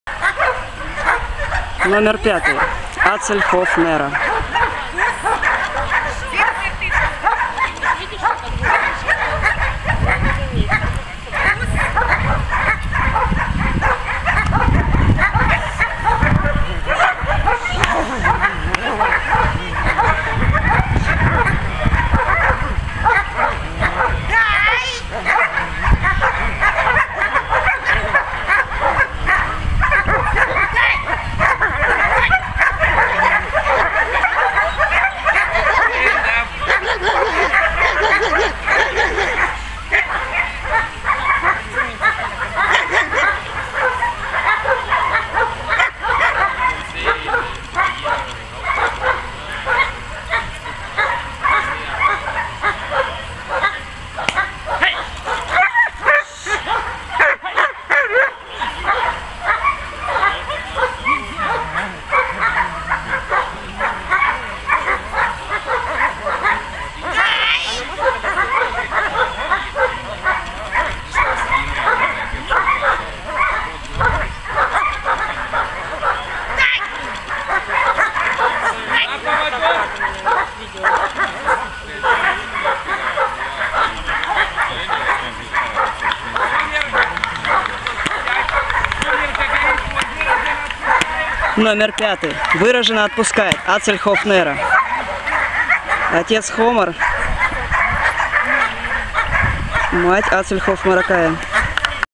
№5 Atsel Hof Nero TSB :: Моно Мариуполь, 23 июня
6 ОТЛИЧНО. ВЫРАЖЕНО/ОТПУСКАЕТ. Atsel Hof Nero
д.р. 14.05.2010, RKF 2821219, клеймо AHF 0059, чепрачный
о.Homar dei Colli di Uzzano м.Atsel Hof Maraja
зав. Саблина Л., Тамбов вл. Риттер М.Р., Краснодар

Мариуполь, 23 июня
Специализированная выставка немецких овчарок
судья SV Norbert Scharschmidt, владелец питомника "vom Heerbusch"
Помошник судьи по защите - Хусар Александр.